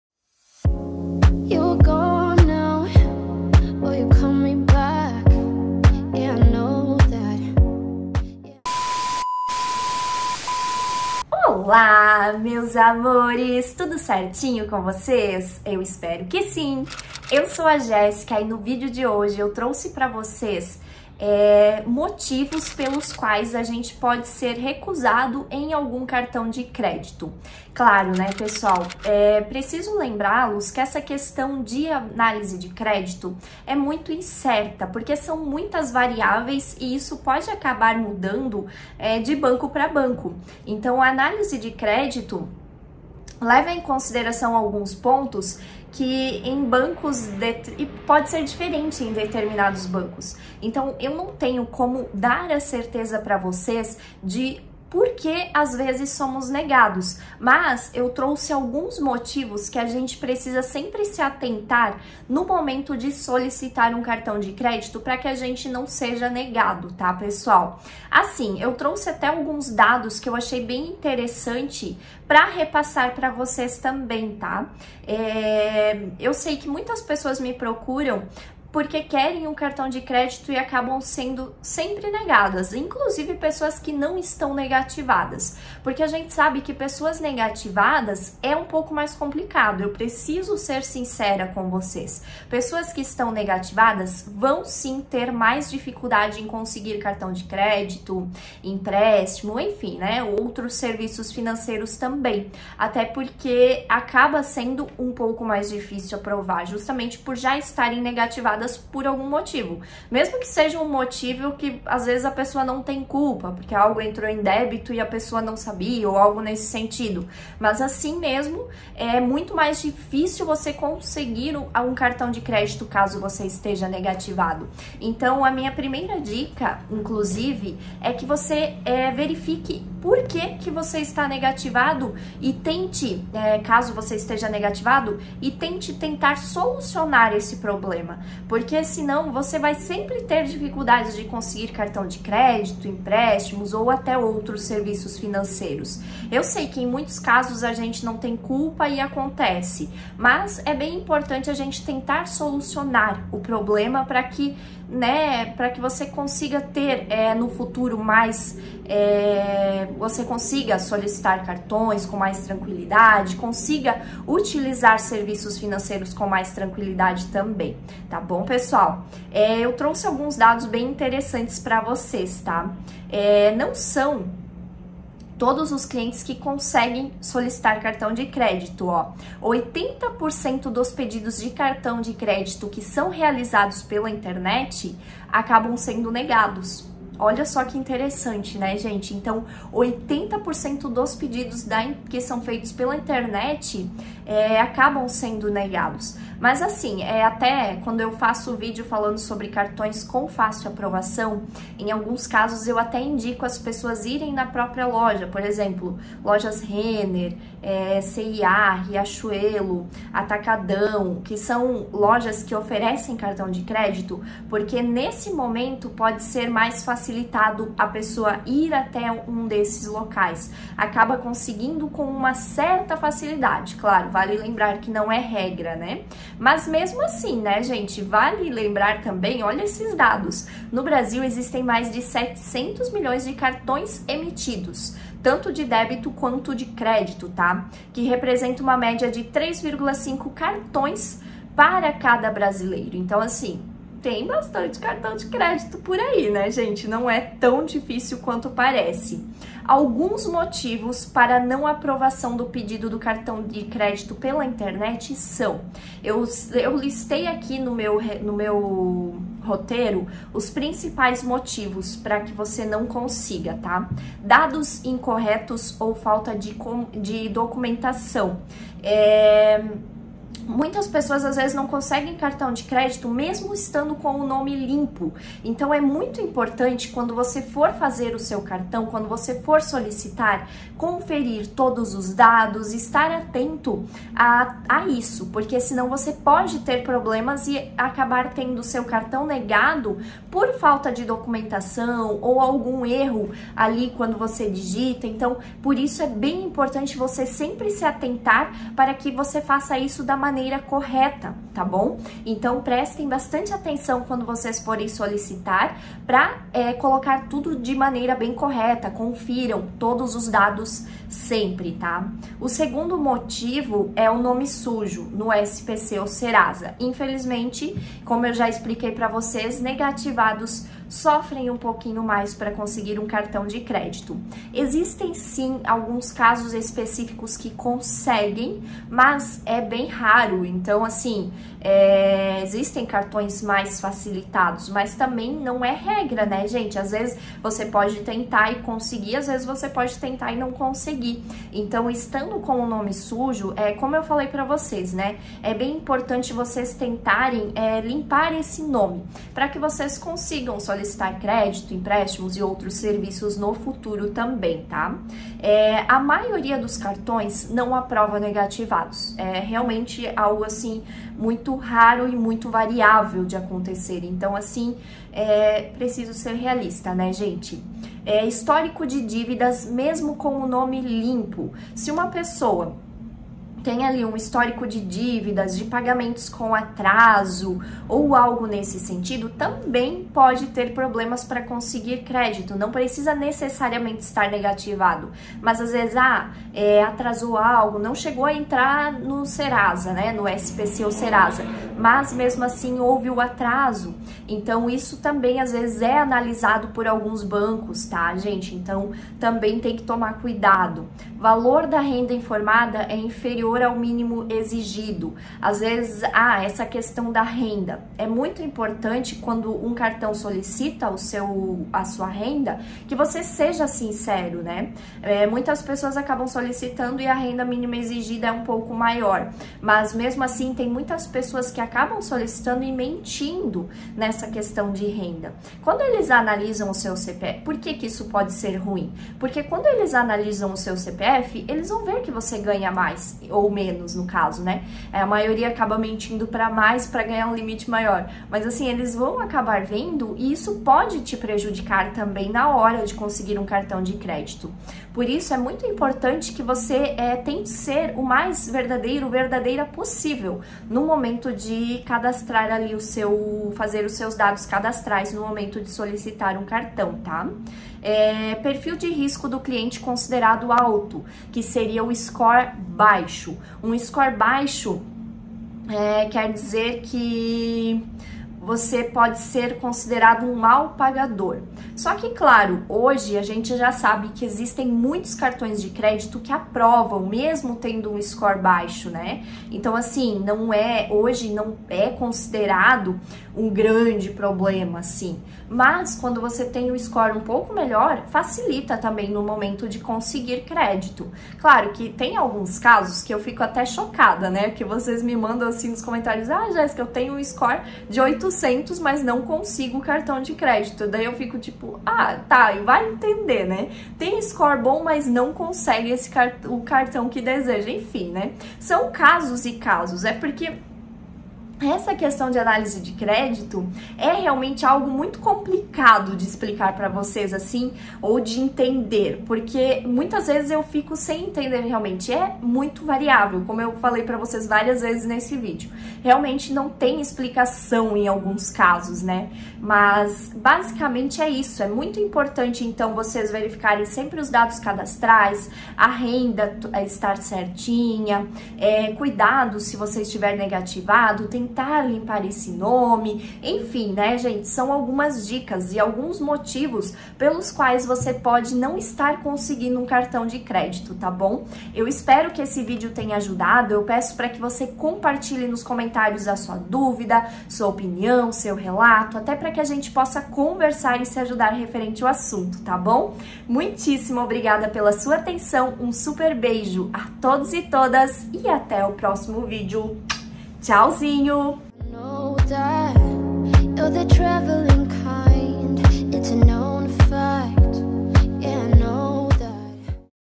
CARTÃO de CRÉDITO | Porque NÃO CONSIGO APROVAÇÃO?
No vídeo de hoje vim conversar com vocês a respeito de um assunto que vocês tem bastante dúvidas que é a aprovação em cartão de crédito. Trouxe alguns motivos pelos quais você pode não estar conseguindo essa aprovação. Vale lembrar que não é regra pois a análise de crédito acaba sendo algo muito incerto, afinal sempre recebo comentários de vocês falando que tem um score bom mas mesmo assim não conseguem. Realmente acaba sendo muito relativo. De qualquer forma espero que esse vídeo ajude. Peço para que compartilhe sua dúvida, opinião ou relato para que possamos conversar e nos ajudar referente o assunto. Muitíssimo obrigada pela sua atenção, super beijo e até o próximo vídeo.